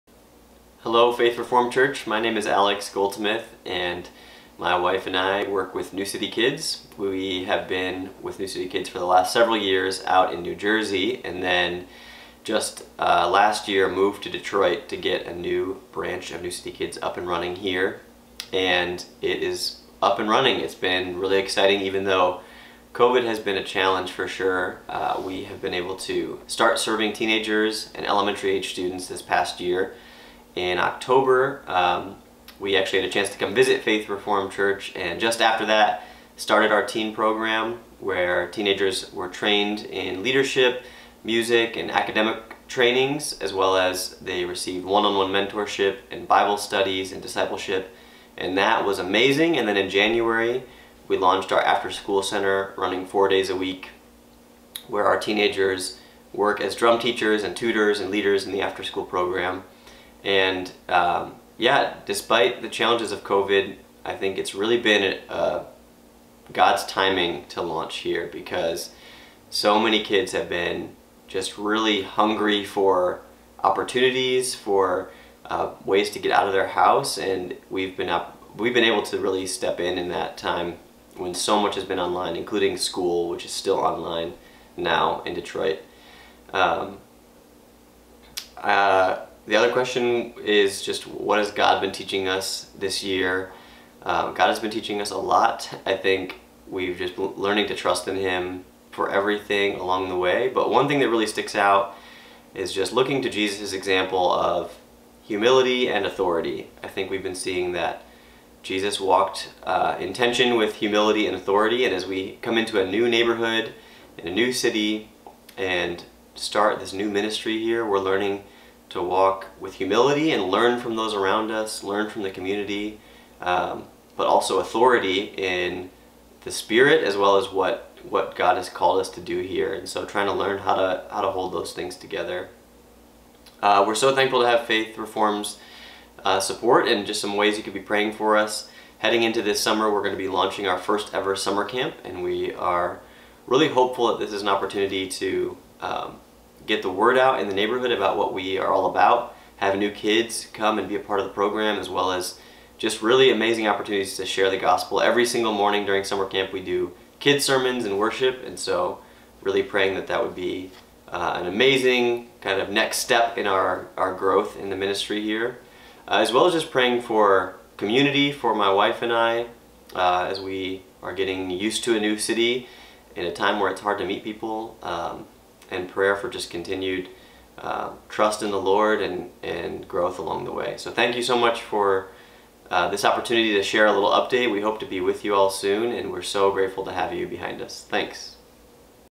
New City Kids — Focus on Missions 2021
Alex Goldsmith provides the update on New City Kids in Detroit